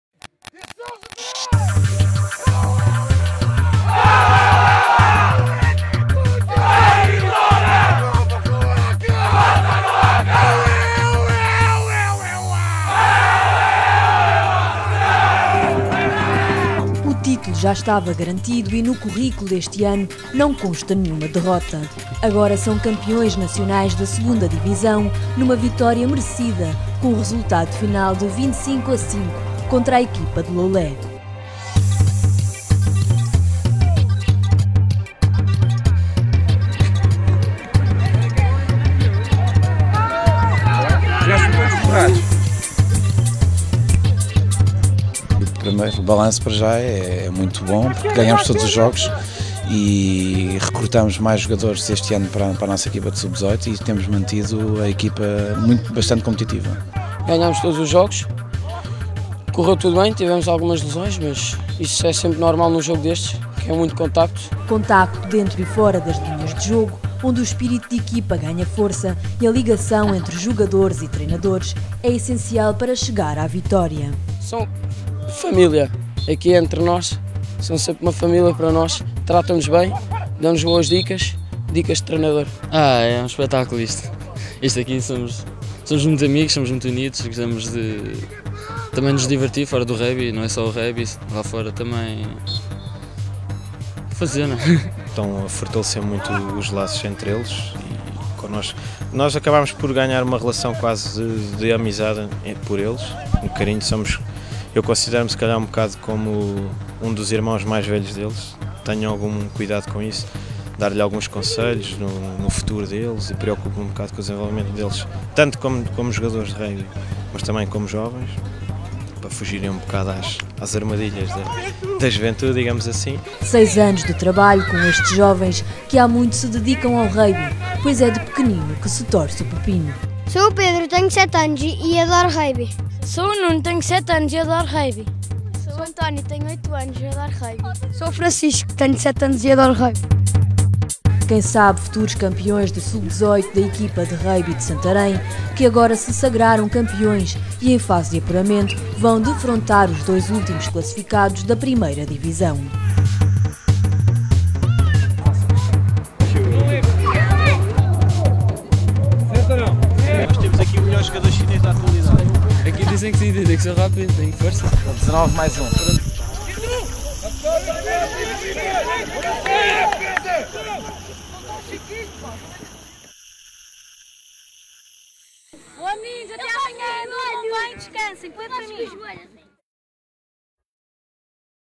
Campeões Nacionais de Rugby são Scalabitanos
Equipa sub-18 do Rugby de Santarém sagraram-se campeões nacionais de 2ª divisão.